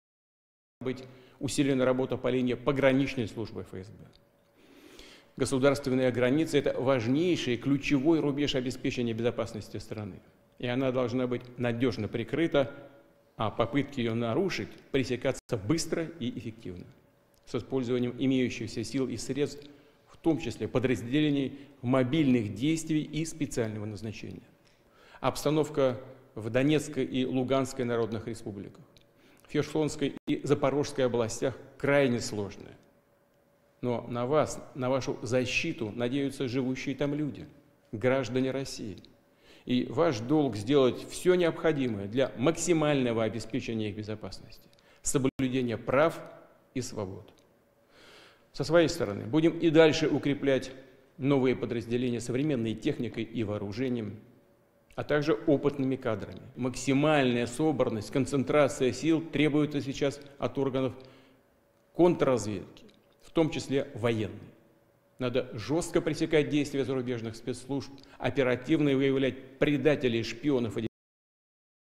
Putin says situation in illegally annexed parts of Ukraine ‘extremely difficult’
Vladimir Putin released a statement on Russia's Security Services Day, ordering the federal authorities to step up surveillance at the country's borders to prevent risks from threats abroad and 'traitors' at home.
 The Russian president goes on to say the situation in the illegally annexed parts of Ukraine, Donetsk and Luhansk, is 'extremely difficult' and ordered security services to ensure the safety of people living there.